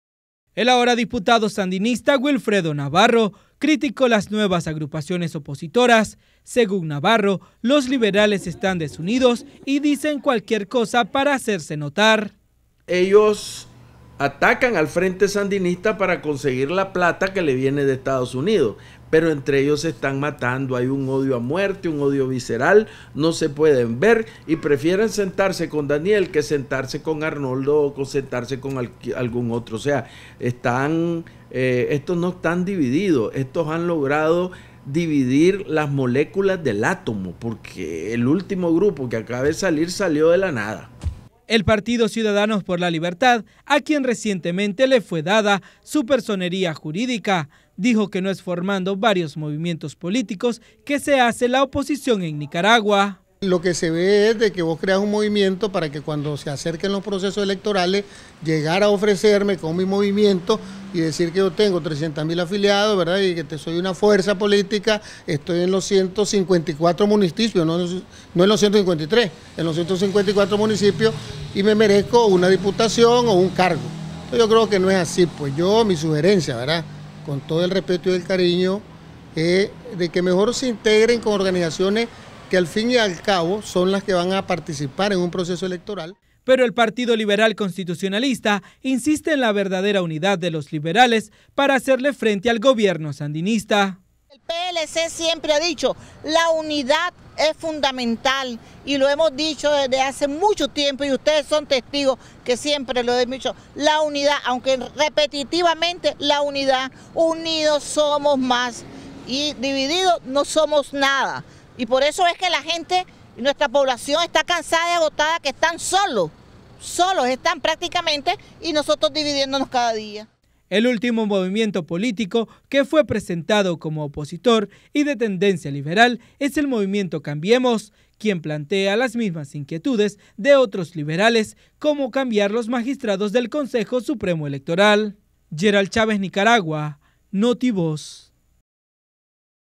Wilfredo Navarro critica a movimientos y partidos políticos que se hacen llamar opositores
El ex liberal Wilfredo Navarro hizo una fuerte crítica a eso movimientos y fuerzas políticas que se autodenominan opositoras.